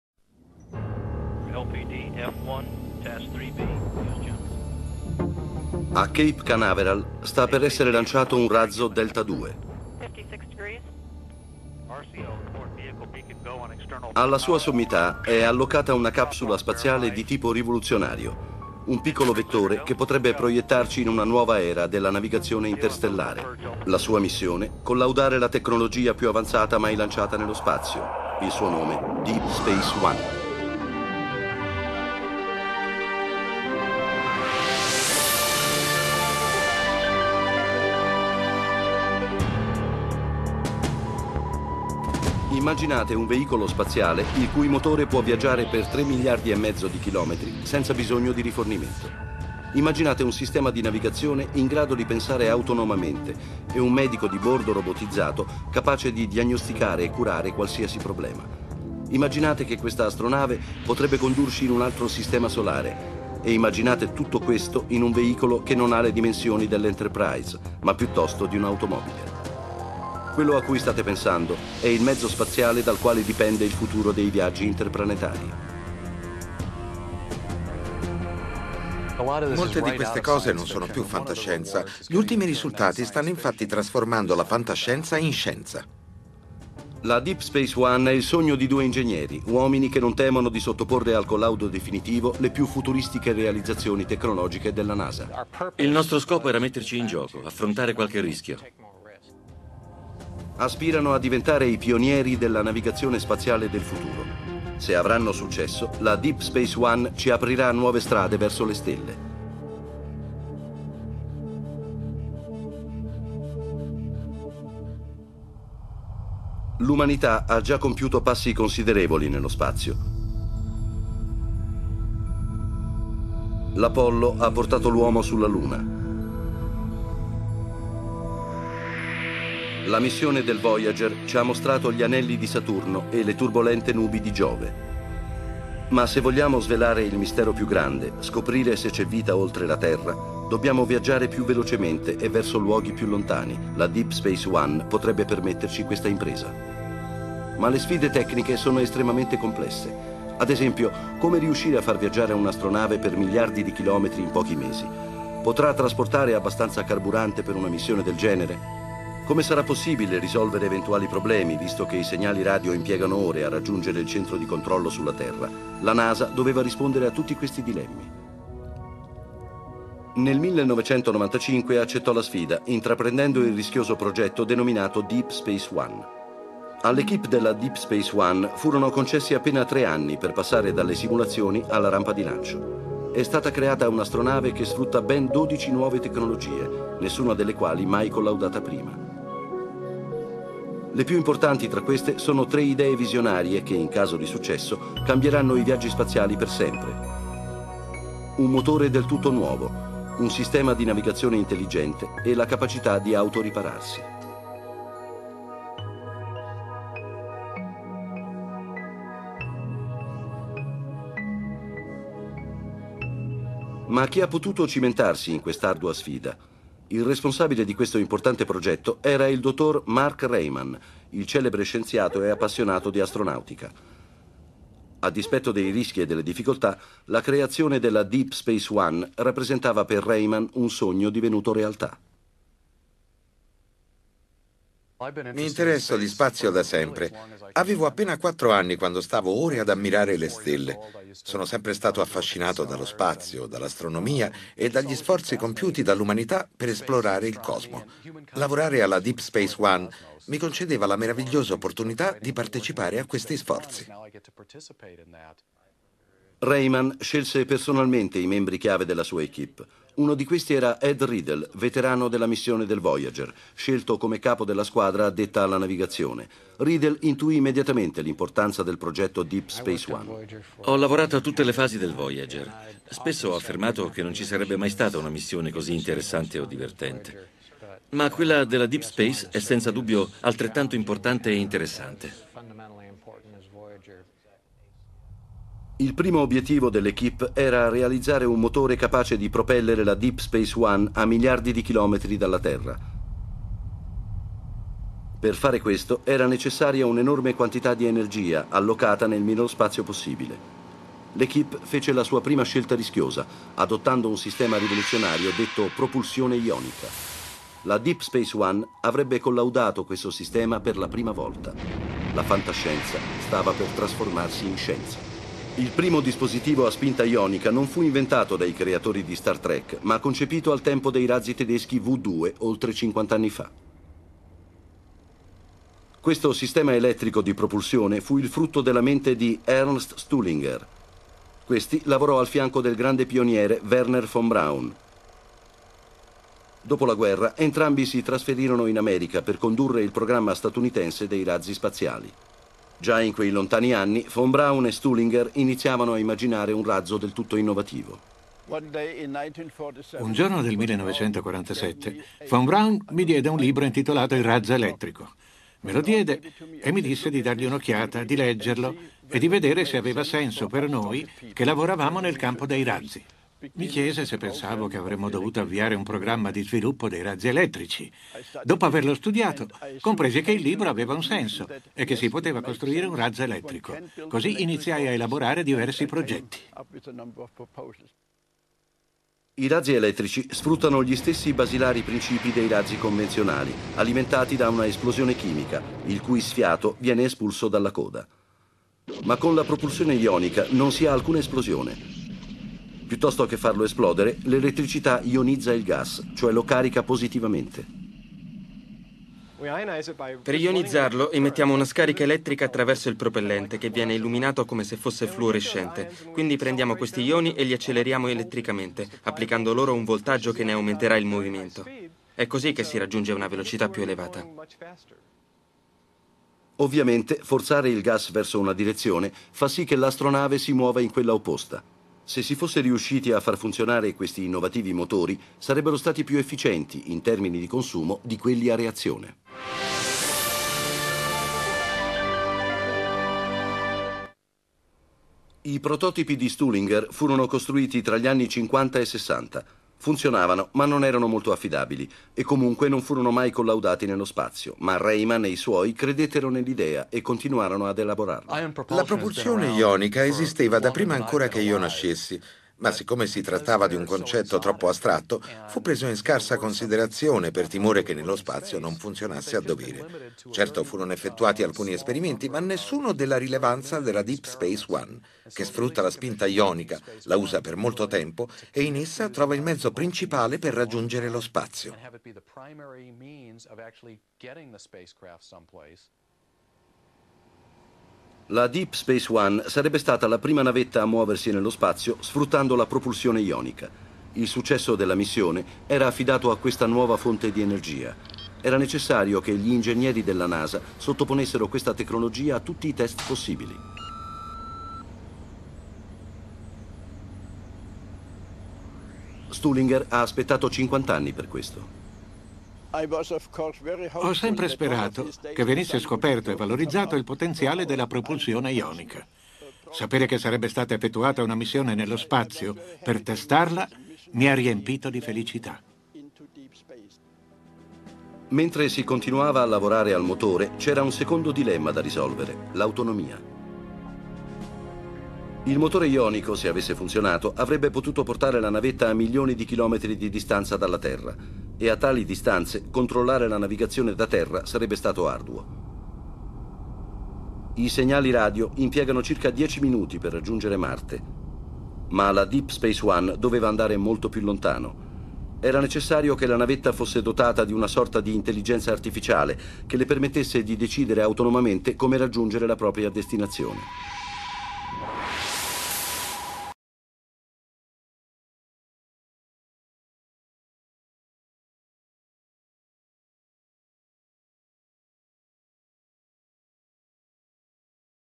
Astronave a propulsione ionica 1/5
Would you like to make some money online by sharing your unused internet connection?

Il futuro ha avuto inizio, un'astronave a propulsione ionica è stata lanciata con successo gettando le basi per le future esplorazioni all'esterno del nostro sistema solare.